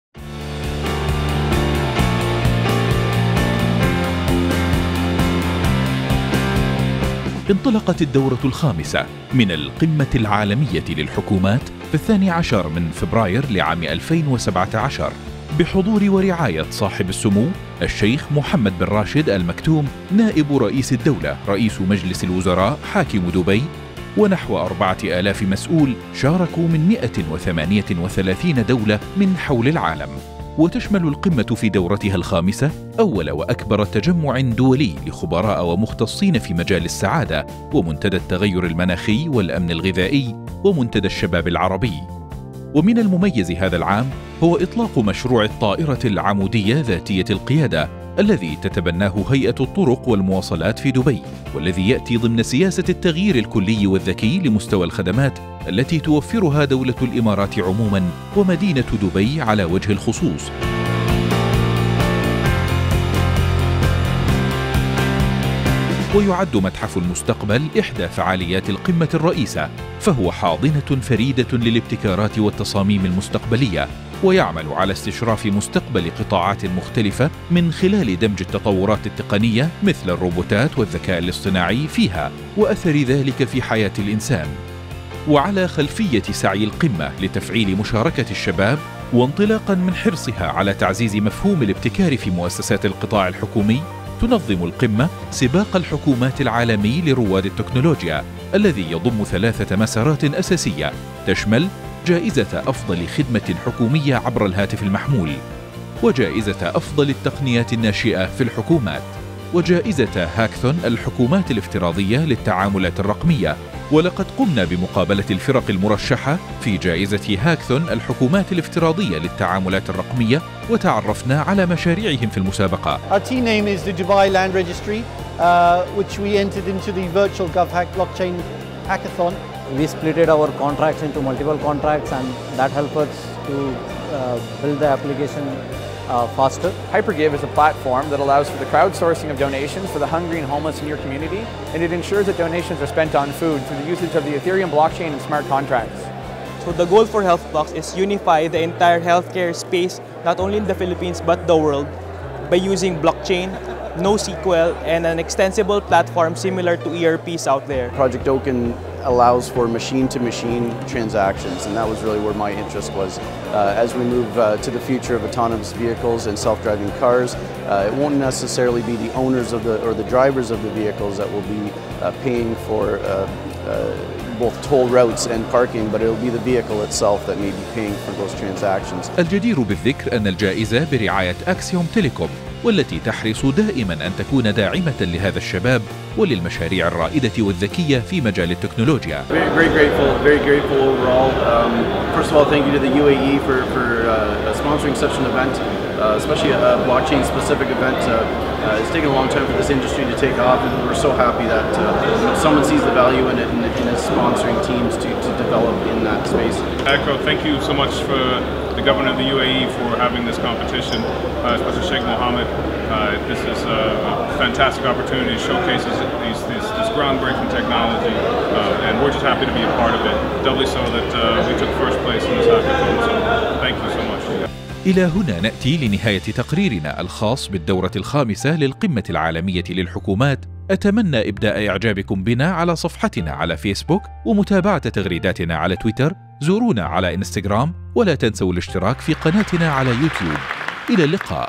World Gov Summit 2017 Report - تقرير القمة العالمية للحكومات ٢٠١٧
نقدم لكم التقرير الكامل للقمة العالمية للحكومات التي اقيمت في دبي في الفترة من ١٢ - ١٤ فبراير ٢٠١٧